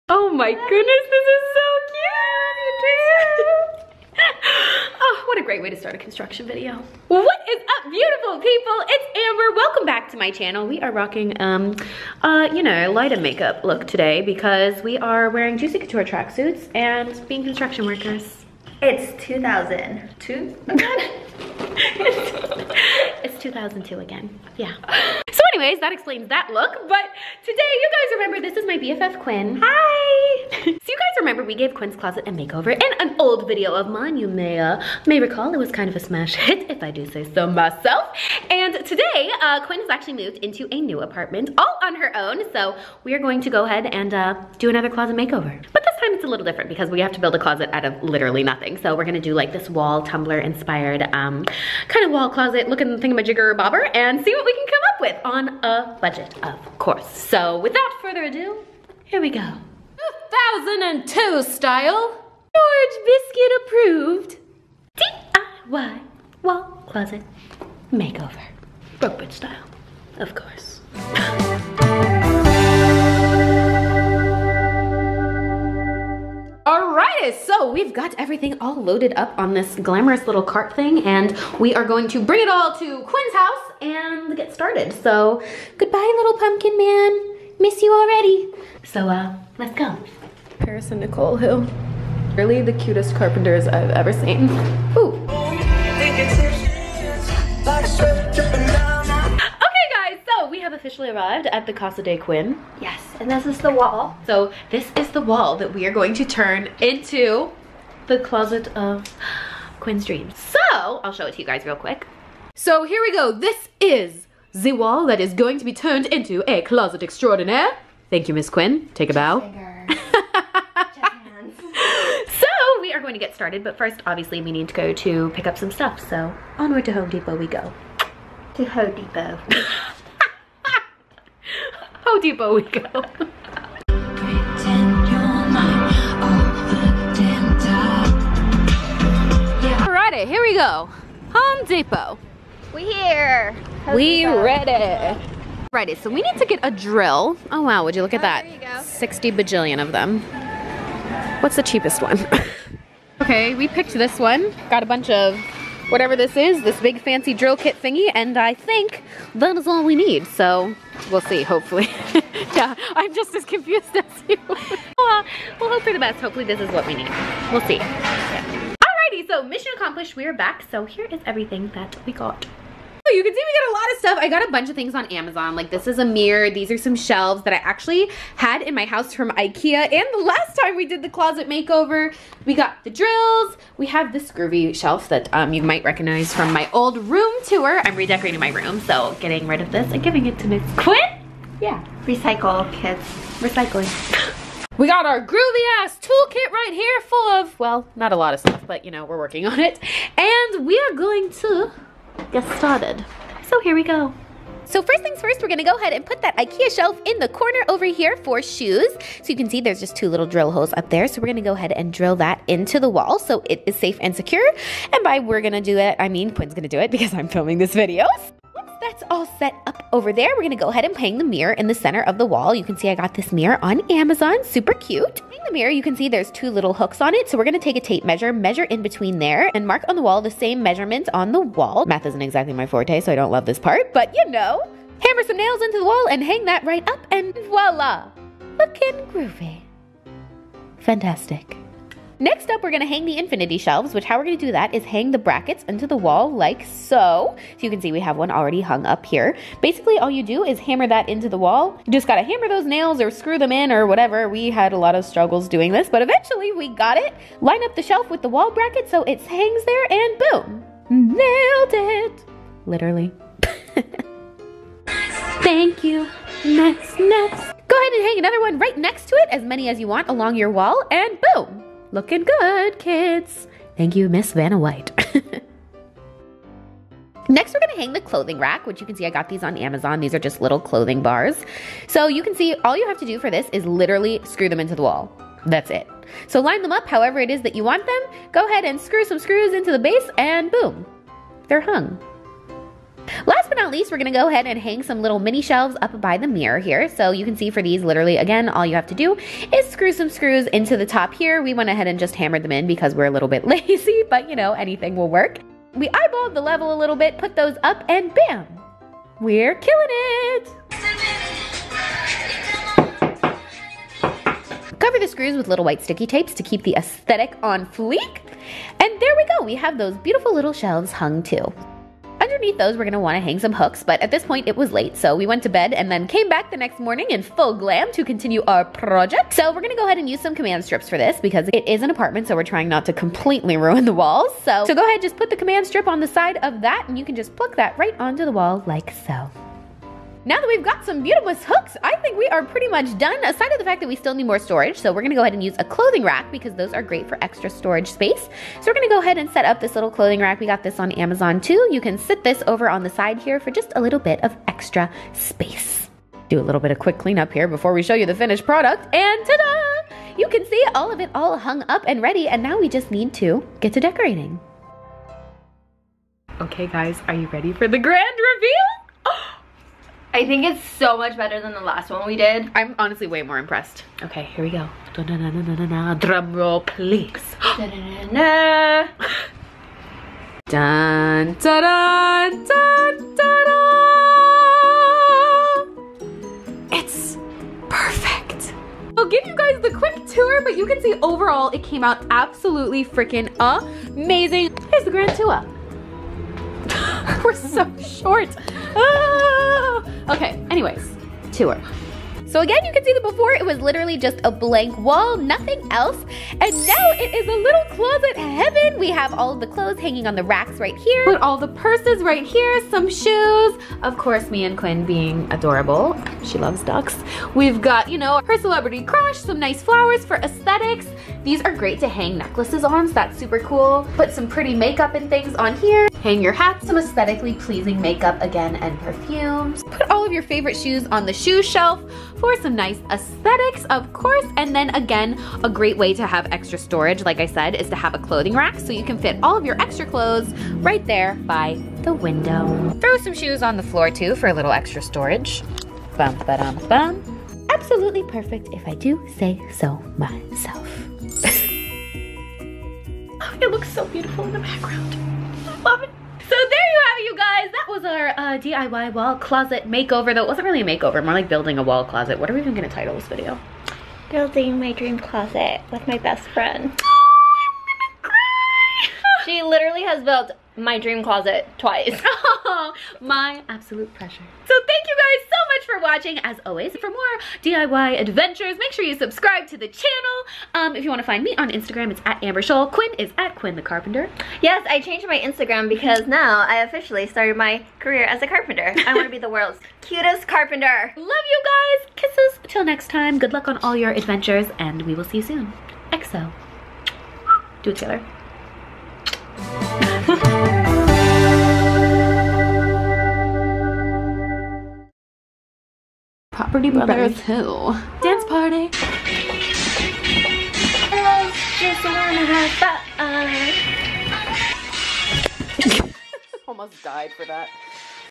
DIY WALL CLOSET MAKEOVER!
HI ANGELS! WE'RE DOING A DIY CLOSET MAKEOVER!!!

My bff Quinn (@QuinnTheCarpenter) just moved into her first new apartment and needs a closet renovation - because she has nothing but a blank wall lol. So we are using my best Ikea hacks and home decor organization ideas to turn her wall into an amazing Tumblr Pinterest inspired closet! So here is our how to guide for making a cheap, easy, fun do it yourself project to create your own dream instagram worthy closet!

...broke bitch style, of course :P

ENJOY THE VLOG TUTORIAL NONSENSE MY LOVES!

2,691,000 kisses!
Xx
Amber :)

CREATE YOUR OWN! (we got the shelves at Ikea, but here are similar options available online from amazon!)

Infinity Shelves
Wall clothing Rack
Shoe shelf
Makeup shelves
Standing clothing rack